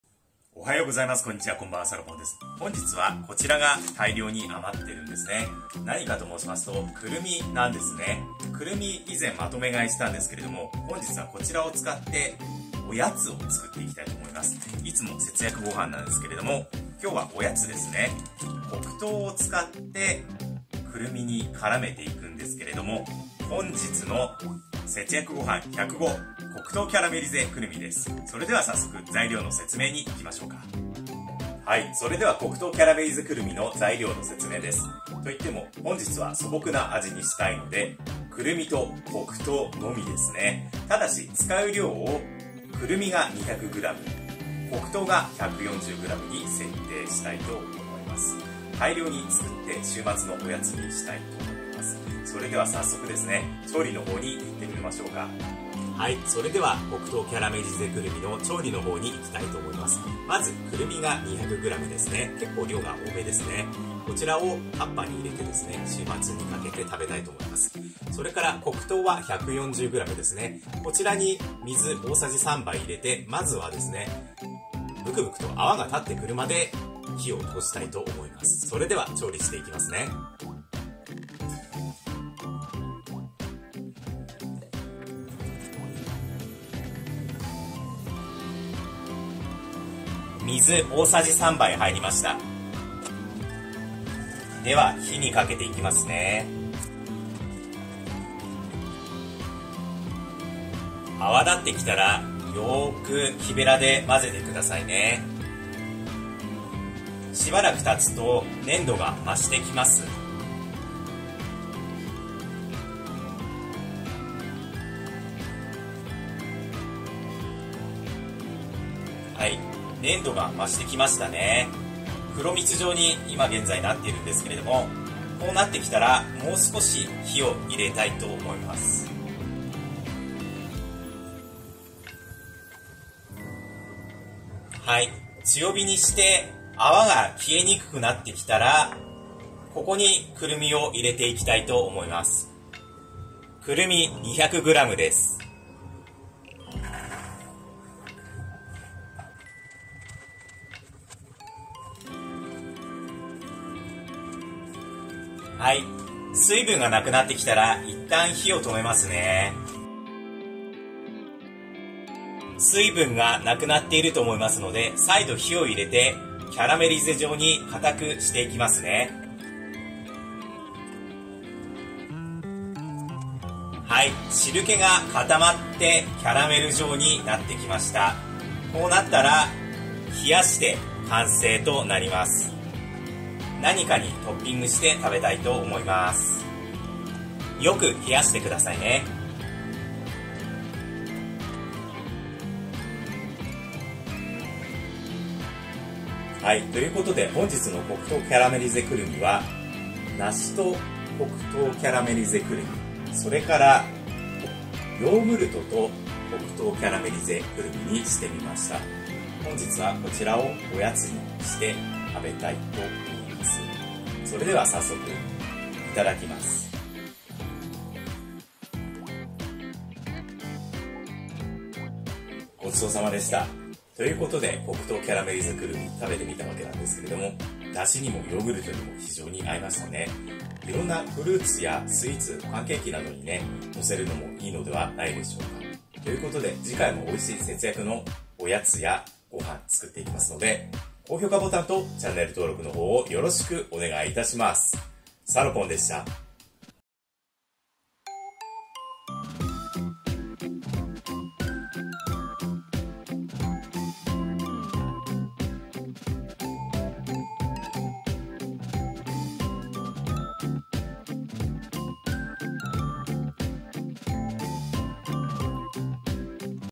素朴なくるみのおやつ 黒糖でキャラメリゼしてヨーグルトと梨に合わせて食べました【節約ごはん105】
素朴な味にしたかったので黒糖とくるみのみです
黒糖とくるみの甘さを感じてくださいね